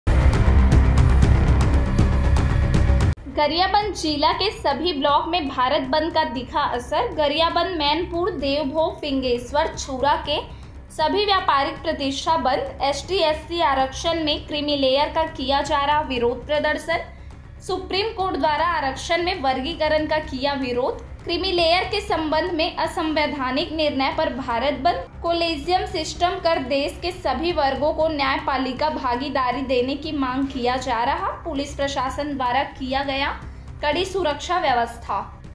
Breaking News: Gariyaband ब्लाकों में दिखा बंद का असर SC ST आरक्षण में किम्री लेयर का विरोध प्रदर्शन
Breaking News: Effect of bandh visible in Gariyaband blocks, Kimri layer protest in SC ST reservation